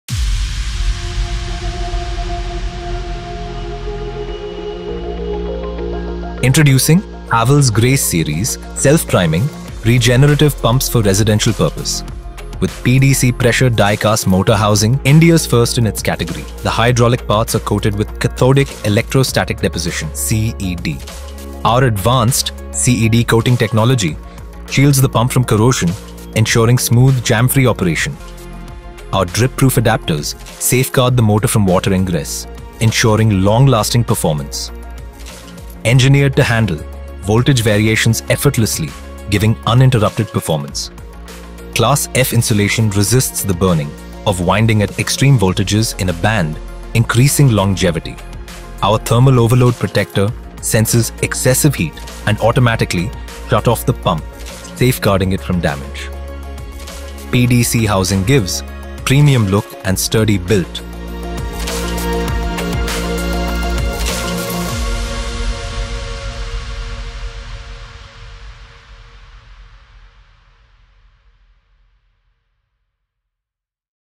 Havells Grace Pump | India’s First PDC Motor Housing Pump
Discover the revolutionary Havells Grace Series - Self-Priming Regenerative Pump, India’s first in its category with PDC Motor Housing for unmatched durability and performance.

With advanced CED coating to prevent corrosion, drip-proof adaptors for motor protection, and the ability to handle voltage variations effortlessly, the Grace Pump ensures smooth, long-lasting performance. Experience reliability, efficiency, and innovation - all in one!